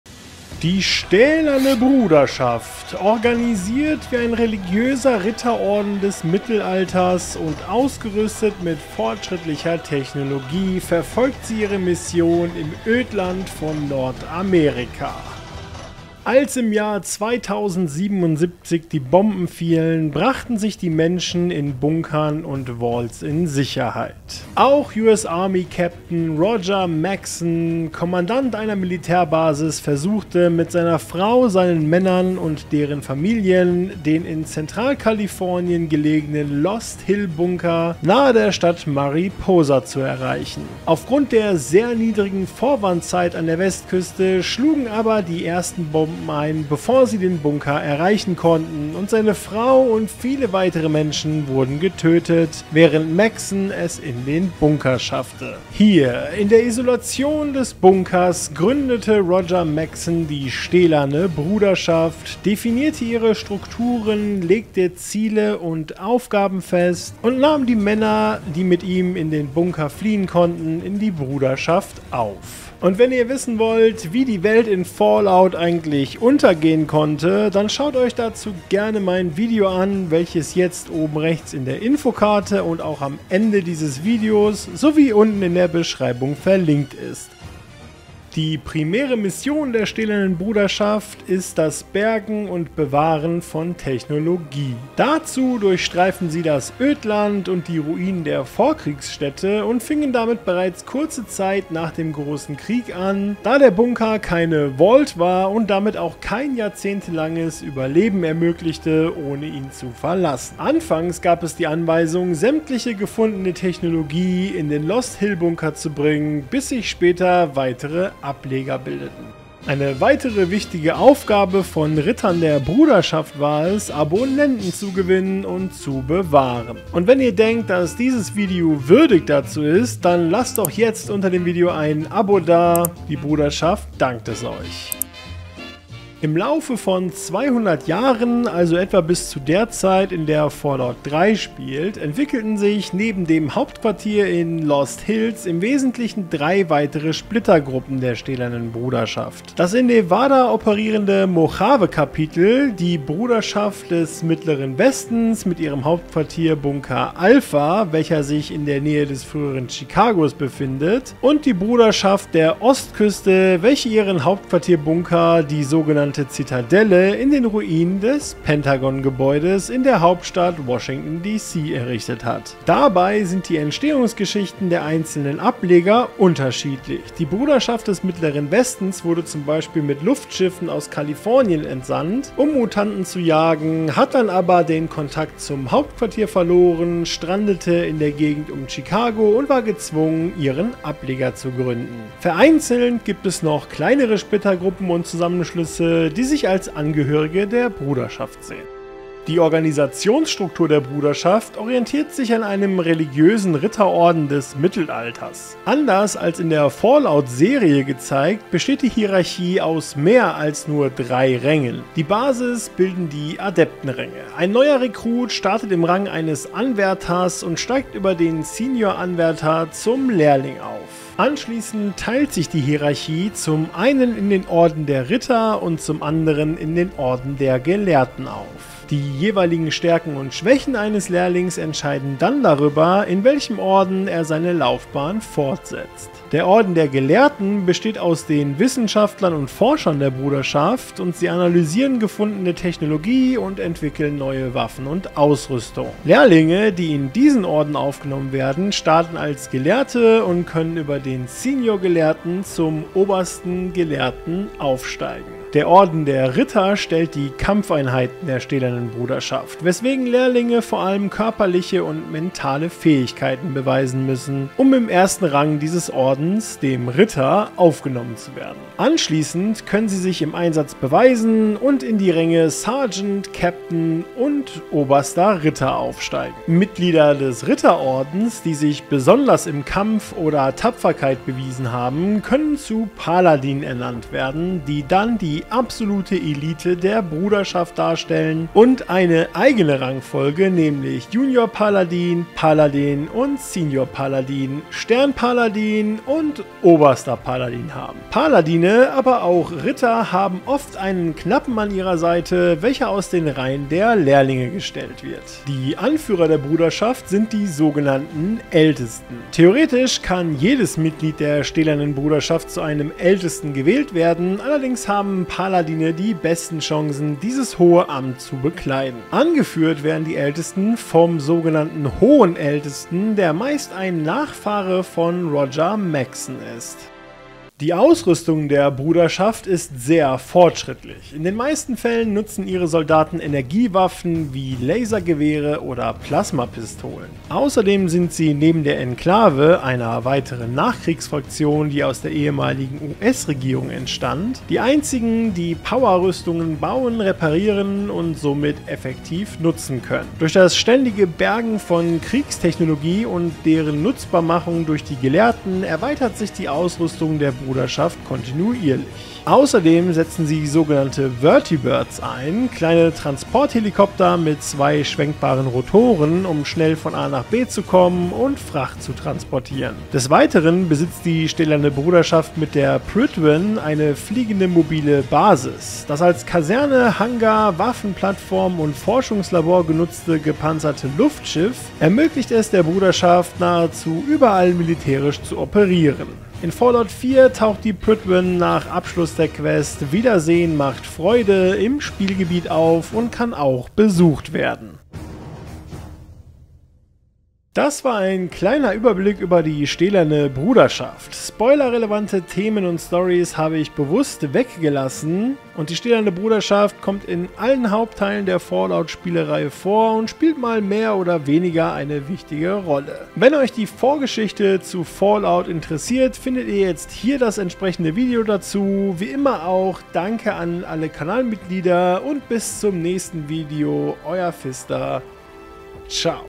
Die Geschichte der Stählernen Bruderschaft aus Fallout
Die Stählerne Bruderschaft Lore kommt in allen Hauptteilen der Fallout Spielereihe und auch in der Serie vor. Heute zeige ich euch, wie Bruderschaft entstanden ist und was sie in der Fallout Welt ausmacht